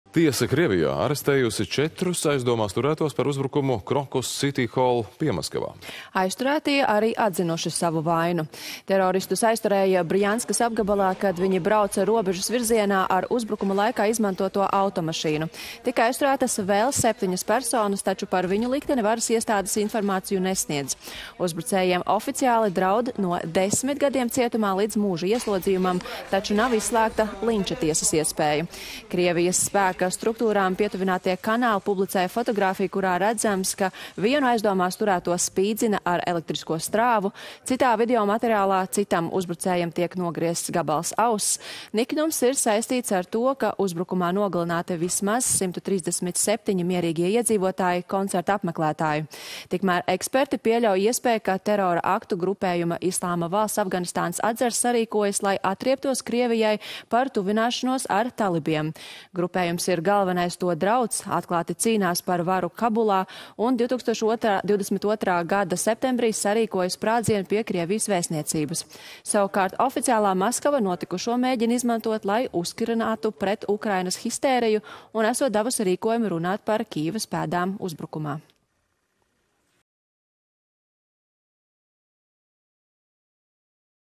Par teroraktu Krievijas koncertzālē apcietina četrus cilvēkus
Tiesa Krievijā arestējusi 4 aizdomās turētos par teroraktu netālu no Maskavas esošajā koncertzālē "Crocus City Hall". Aizturētie arī atzinuši savu vainu.

Tika aizturētas vēl 7 personas, taču par viņu likteni varasiestādes informāciju nesniedz.

Uzbrucējiem oficiāli draud no 10 gadiem cietumā līdz mūža ieslodzījumam, taču nav izslēgta linča tiesas iespēja.